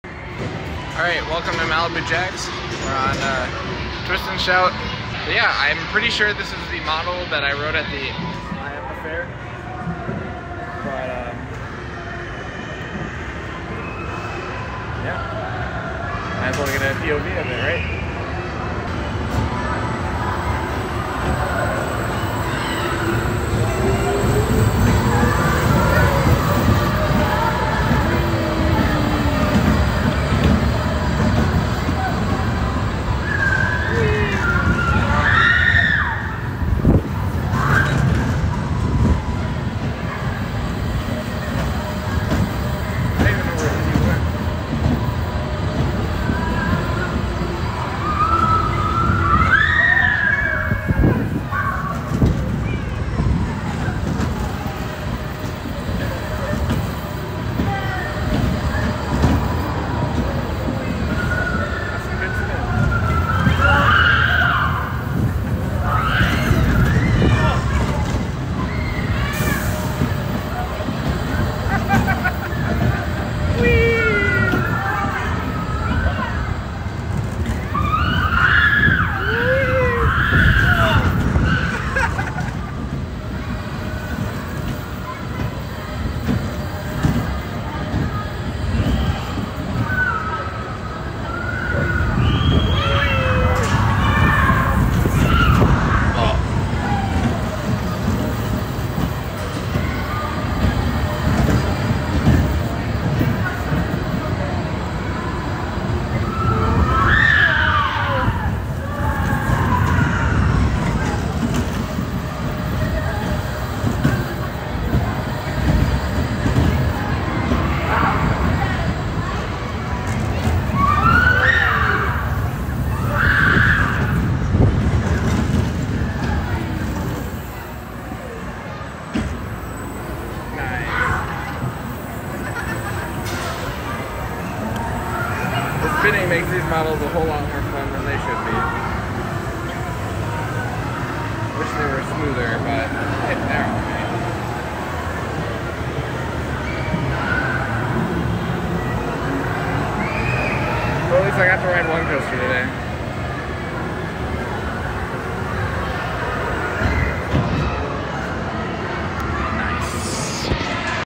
Twist-n-Shout POV Malibu Jack’s Quad Cities
It’s a B model SBF spinner!